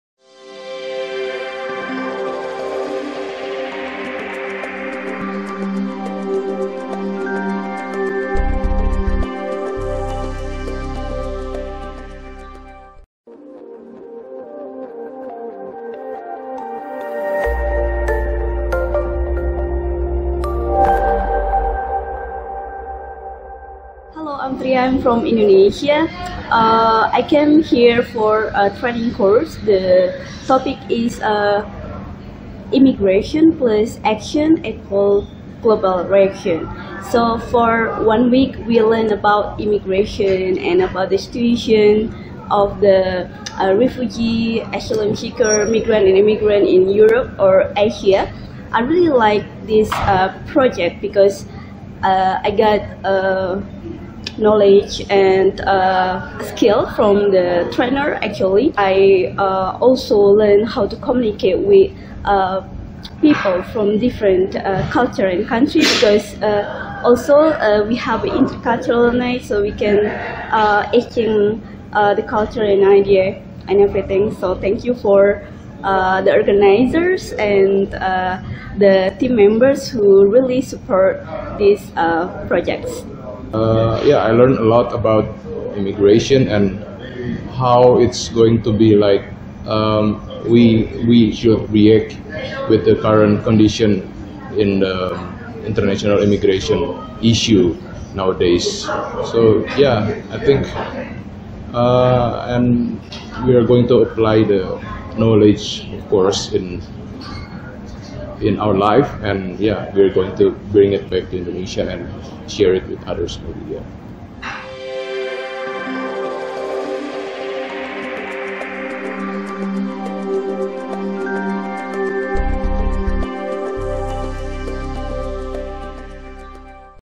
An interview with our colleagues, Tria and Hermanto!
Tria and Hermanto are 2 people from Indonesia, who are interesed in the global migration problem. They are also concerned about the refugee crises around the world and this is why they participate in the project, CBY I/migration + action = Global reaction, for which they were on the second mobility meeting- a training course in Madrid, in the period 10-16. December 2017.